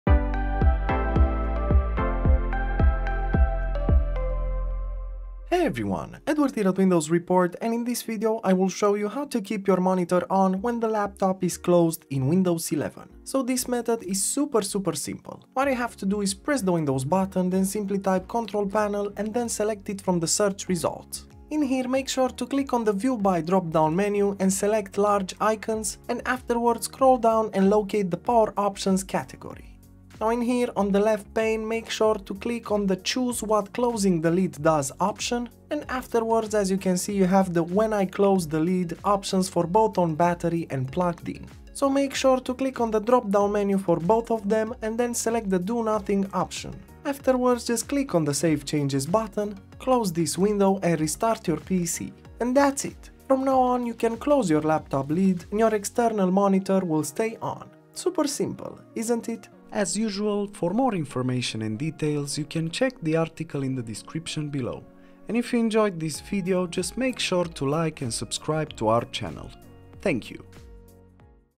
How to Keep your Monitor on when the Laptop is Closed in Windows 11
| ⚙️Try Fortect to fix and optimize your PC like a Pro

Interested in How to Keep your Monitor on when the Laptop is Closed in Windows 11?
Here's how you can do it! Closing the lid is super cool if you want to save space and if you have an external monitor. In this way you can optimize your setup for a modern look and more efficiency!

0:00 - Intro
0:09 - Use Settings